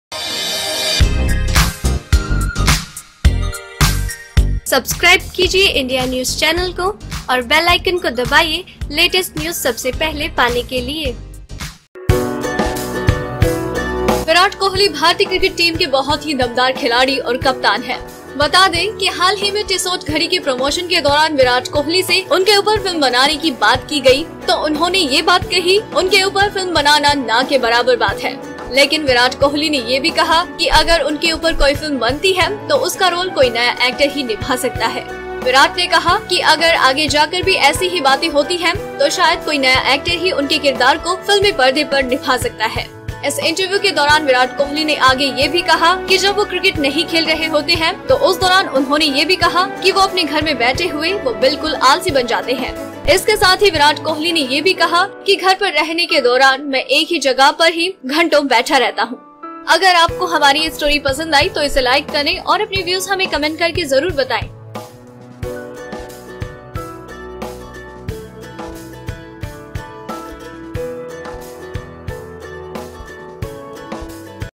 विराट कोहली ने कहा, सिर्फ यह एक्टर निभा सकता है उनकी बायोपिक में रोल, देखें विडियो
विराट कोहली भारतीय क्रिकेट टीम के बहुत ही दमदार खिलाड़ी और कप्तान हैं। बता दें कि हाल ही में TISSOT घड़ी के प्रमोशन के दौरान विराट कोहली से उनके ऊपर फिल्म बनाने की बात की गई तो उन्होंने यह बात कही कि उनके ऊपर फिल्म बनाना न के बराबर बात है।देखें विडियो

Edit By -